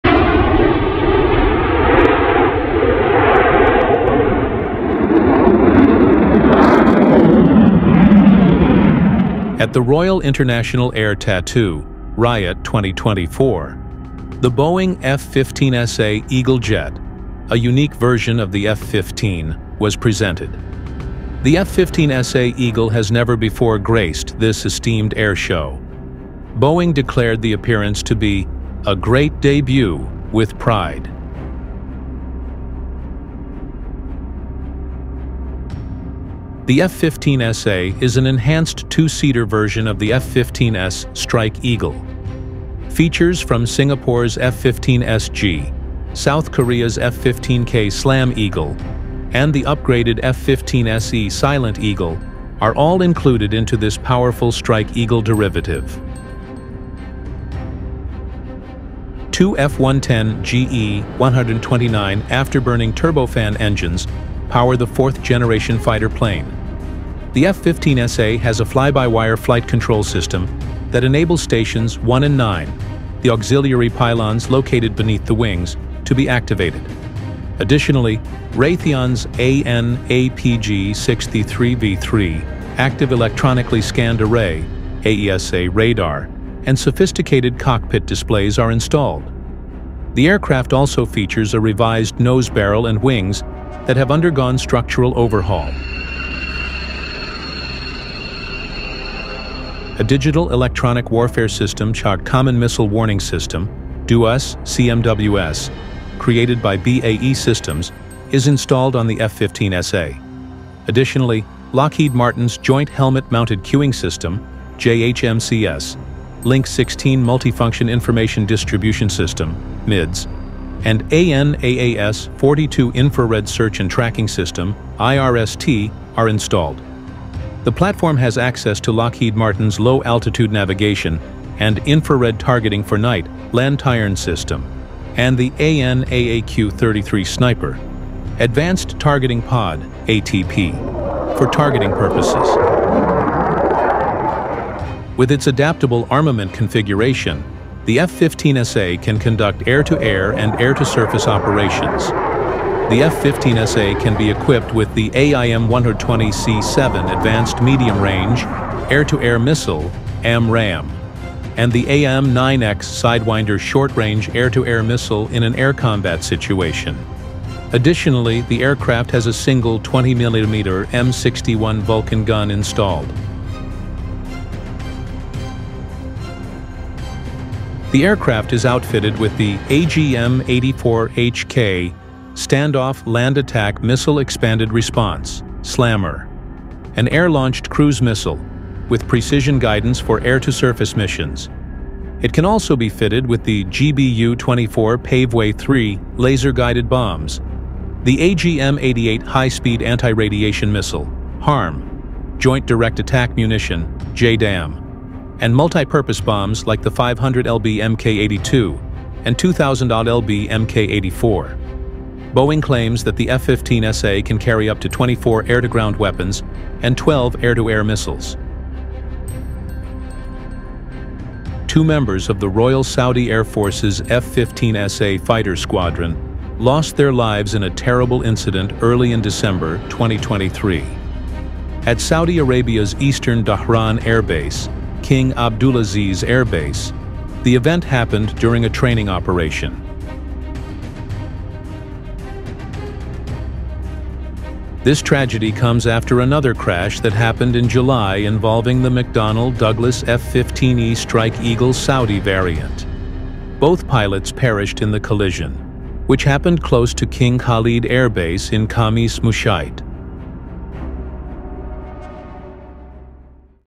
CRAZY... !!!! Upgraded Boeing F-15SA Eagle fighter jet makes its debut at UK air show.
At the Royal International Air Tattoo [RIAT] 2024, the Boeing F-15SA Eagle jet—a unique version of the F-15—was presented. The F-15SA Eagle has never before graced this esteemed air show. Boeing declared the appearance to be "a great debut!" with pride.

The F-15SA is an enhanced two-seater version of the F-15S Strike Eagle. Features from Singapore's F-15SG, South Korea's F-15K Slam Eagle, and the upgraded F-15SE Silent Eagle are all included into this powerful Strike Eagle derivative.

Two F110-GE-129 afterburning turbofan engines power the fourth generation fighter plane. The F-15SA has a fly-by-wire flight control system that enables stations one and nine, the auxiliary pylons located beneath the wings, to be activated. Additionally, Raytheon's AN/APG-63[V]3 active electronically scanned array [AESA] radar and sophisticated cockpit displays are installed. The aircraft also features a revised nose barrel and wings that have undergone structural overhaul.

A digital electronic warfare system/common missile warning system (DEWS/CMWS) created by BAE Systems is installed on the F-15SA. Additionally, Lockheed Martin's Joint Helmet Mounted Cueing System [JHMCS], Link-16 multifunction information distribution system [MIDS], and AN/AAS-42 infrared search and tracking system [IRST] are installed. The platform has access to Lockheed Martin's Low Altitude Navigation and Infrared Targeting for Night [LANTIRN] system and the AN/AAQ-33 Sniper advanced targeting pod [ATP] for targeting purposes.

With its adaptable armament configuration, the F-15SA can conduct air-to-air and air-to-surface operations. The F-15SA can be equipped with the AIM-120C-7 Advanced Medium Range Air-to-Air Missile [AMRAAM] and the AIM-9X Sidewinder short-range air-to-air missile in an air combat situation. Additionally, the aircraft has a single 20 mm M61 Vulcan gun installed.

The aircraft is outfitted with the AGM-84H/K Stand-off Land Attack Missile-Expanded Response [SLAM-ER], an air-launched cruise missile with precision guidance for air-to-surface missions. It can also be fitted with the GBU-24 Paveway III laser-guided bombs, the AGM-88 High-speed Anti-Radiation Missile [HARM], Joint Direct Attack Munition [JDAM], and multipurpose bombs like the 500 lb Mk.82 and 2,000 lb Mk.84. Boeing claims that the F-15SA can carry up to 24 air-to-ground weapons and 12 air-to-air missiles.

Two members of the Royal Saudi Air Force's F-15SA fighter squadron lost their lives in a terrible incident early in December 2023. At Saudi Arabia's eastern Dhahran Air Base, King Abdulaziz Air Base, the event happened during a training operation.

This tragedy comes after another crash that happened in July involving the McDonnell Douglas F-15E Strike Eagle Saudi variant. Both pilots perished in the collision, which happened close to King Khalid Air Base in Khamis Mushait.